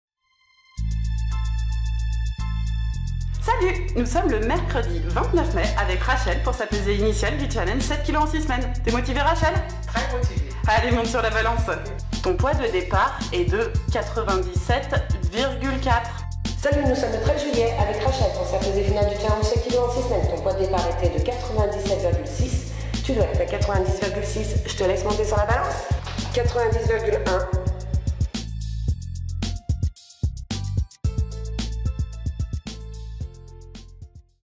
Transformation physique rapide Lyon
Rachel a perdu 7,300 kgs en 6 semaines à la Training Academy